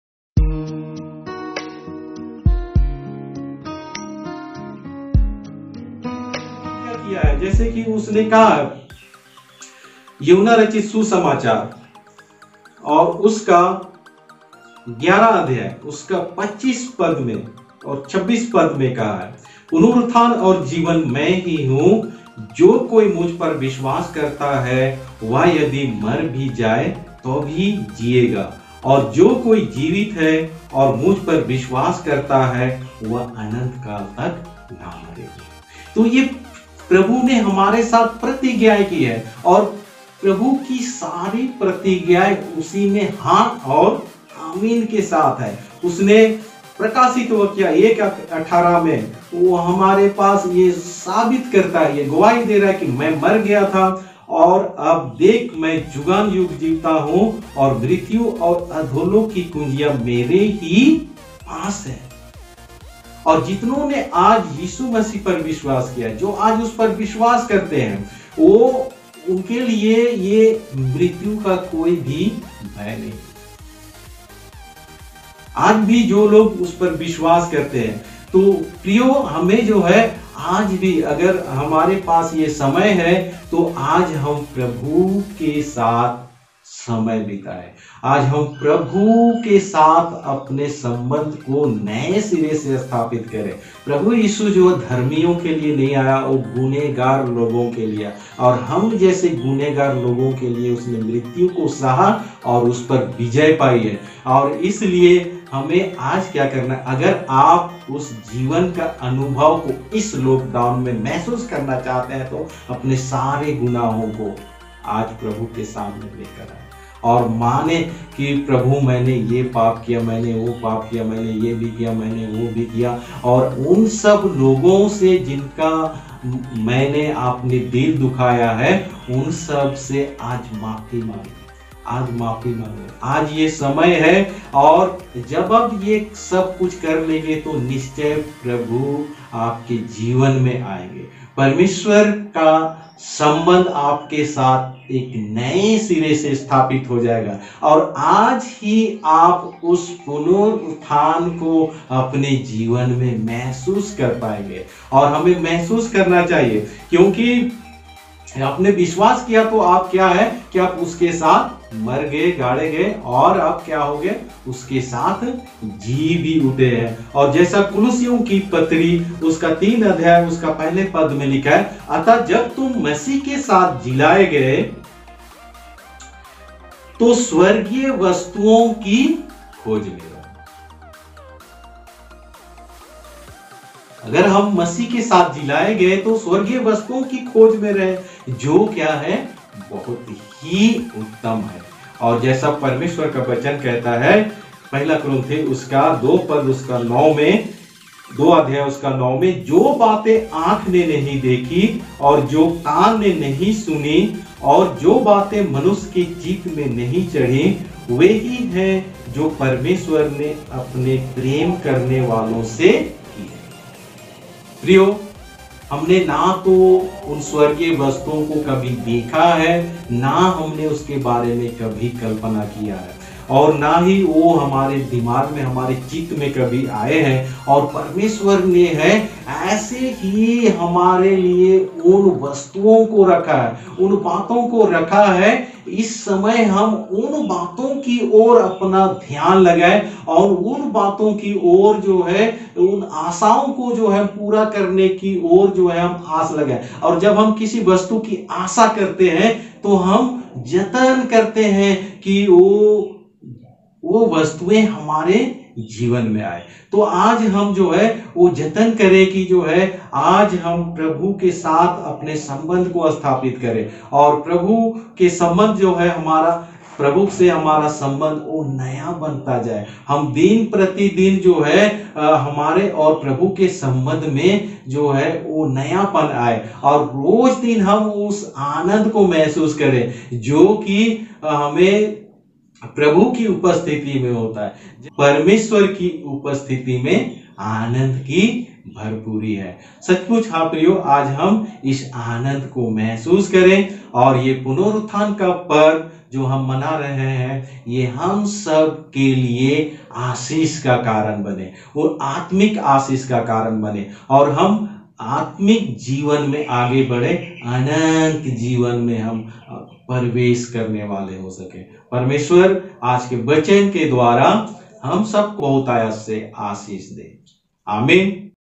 DID U RISEN IN CHRIST (PART-3)EVANGELIST ALBISH KACHHAP || MASIH DE LOG channel
EVANGELIST ALBISH KACHHAP
Did You RISEN in CHRIST
GIVING MESSAGE ON EASTER SUNDAY
FIRE MESSAGE SPIRITUAL MESSAGE SHOWING MIRROR TO ALL OF US
MASIH DE LOG channel